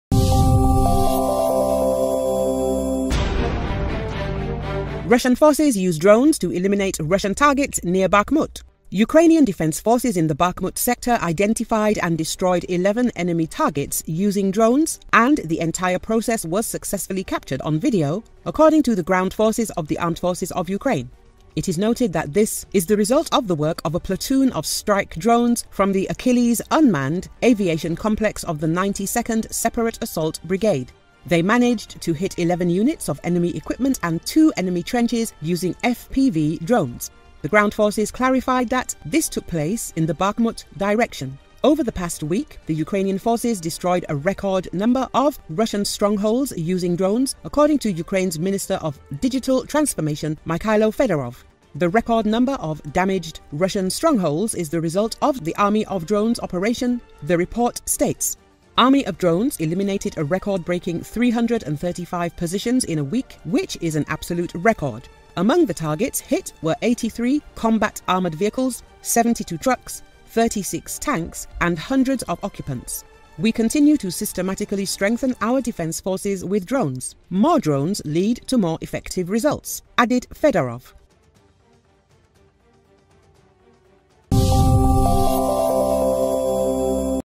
Ukrainian forces use drones to eliminate Russian targets near Bakhmut
Ukrainian defense forces in the Bakhmut sector identified and destroyed 11 enemy targets using drones, and the entire process was successfully captured on video, according to the Ground Forces of the Armed Forces of Ukraine.
It is noted that this is the result of the work of a platoon of strike drones from the ACHILLES unmanned aviation complex of the 92nd Separate Assault Brigade.
They managed to hit 11 units of enemy equipment and 2 enemy trenches using FPV drones.
The Ground Forces clarified that this took place in the Bakhmut direction.
Over the past week, the Ukrainian forces destroyed a record number of Russian strongholds using drones, according to Ukraine's Minister of Digital Transformation, Mykhailo Fedorov.
"The record number of damaged Russian strongholds is the result of the Army of Drones operation," the report states.
Army of Drones eliminated a record-breaking 335 positions in a week, which is an absolute record. Among the targets hit were 83 combat armored vehicles, 72 trucks, 36 tanks, and hundreds of occupants.
"We continue to systematically strengthen our defense forces with drones. More drones lead to more effective results," added Fedorov.